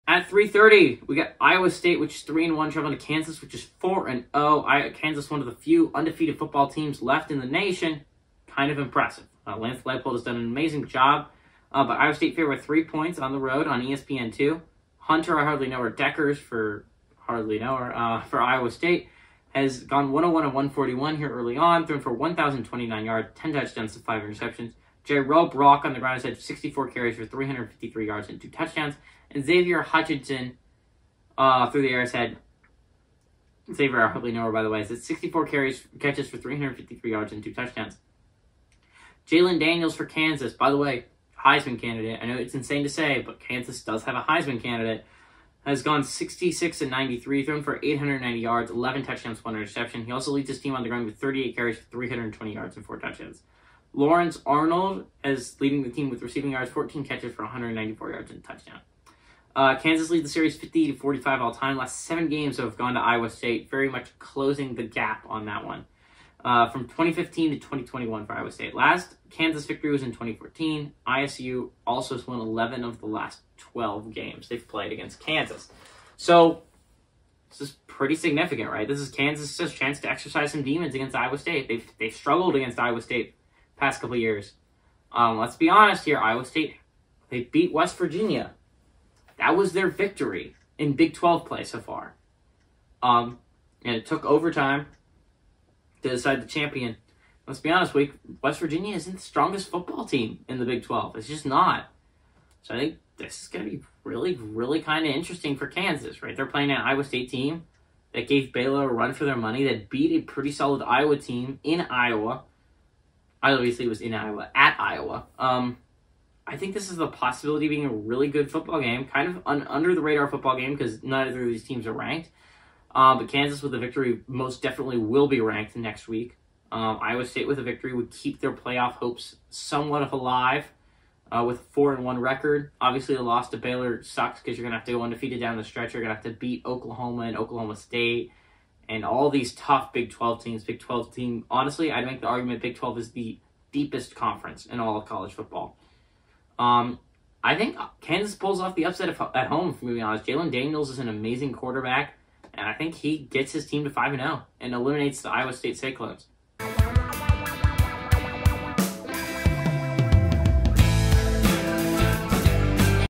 Iowa State @ Kansas
In this video, GGB predicts one of the biggest games of the week. Will the Cyclones get a bounce back win or will the Jayhawks stay undefeated? The only way to know is to watch the video.